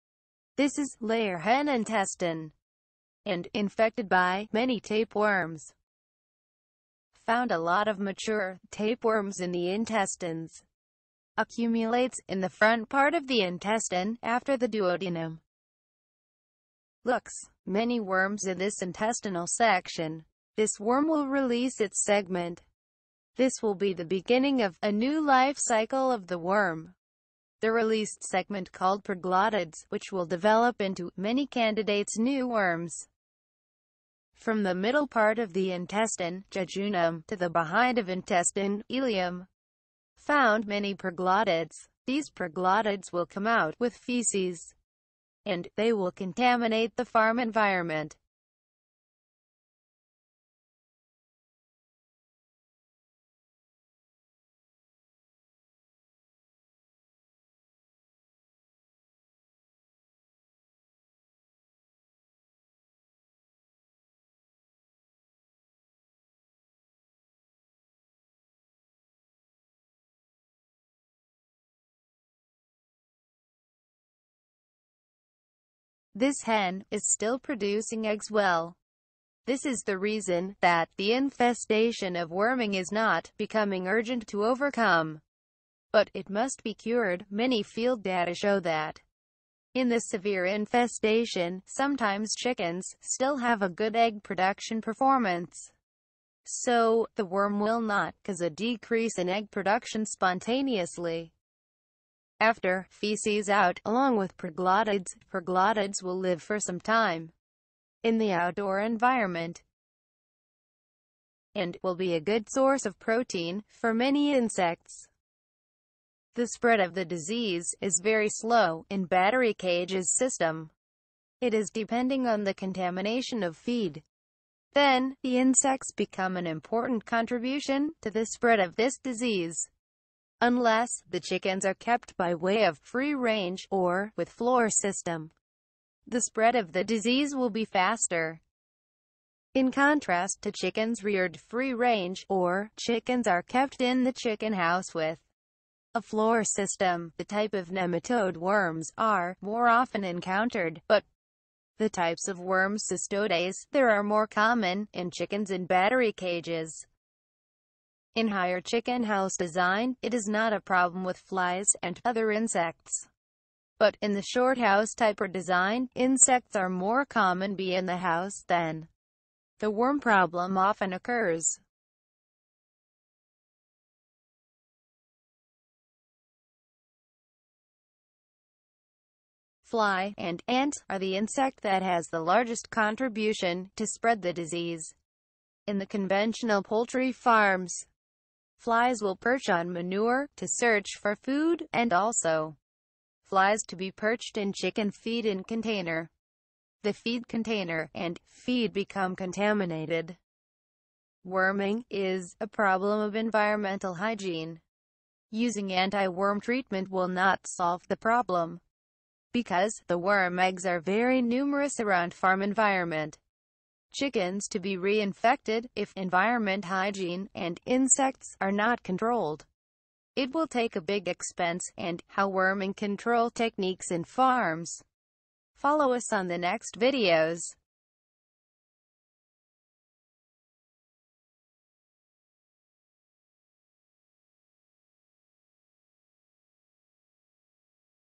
Tapeworms in Chickens, Common in Poultry Ranch, Vet Learning videos
See video, Severe tapeworm infestation in chicken (laying hen), but the hen still produce eggs weel.
Common worms in poultry ranch. Distance Learning Materials for Veterinary college.

tapeworms in chickens commonly occur in laying chickens with battery cages system.  Chickens are not directly in contact with proglottids, but the insects become a carrier of the mechanical carrier to spread of this worm disease in chickens.

Hopefully, this is a topic of discussion, it became a research idea for students of veterinary medicine, vet tech, and veterinarians. Learn not only in schools but also from the field.